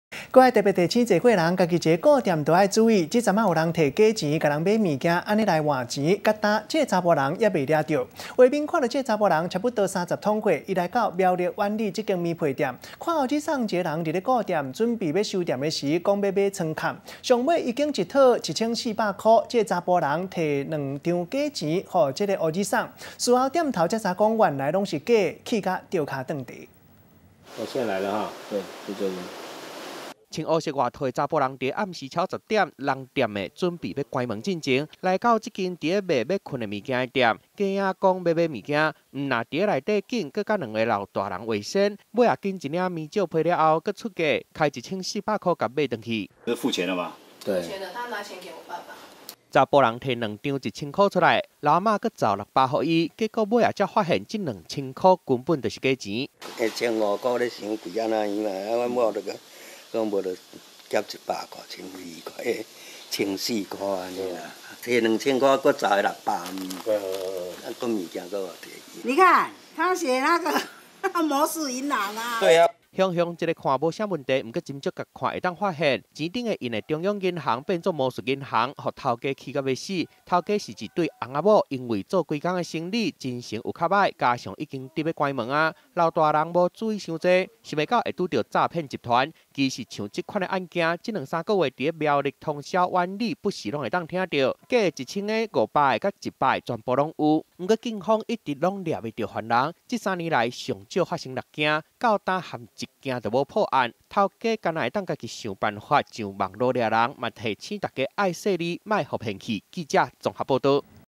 佯裝買涼被男殺價付2千偽鈔詐寢具行
提醒濟歲人家己一e顧店愛注意，有人提假錢共人買物件，閣換錢，一e差不多三十歲左右的男子，去苗栗苑裡一間綿被店，看著歐吉桑一e人佇顧店，準備欲收店的時，講欲買床蓋，上尾揀一套一千四百元，查埔人提兩張假錢予伊，事後店頭才發現攏是假的，氣甲tio跤tng蹄。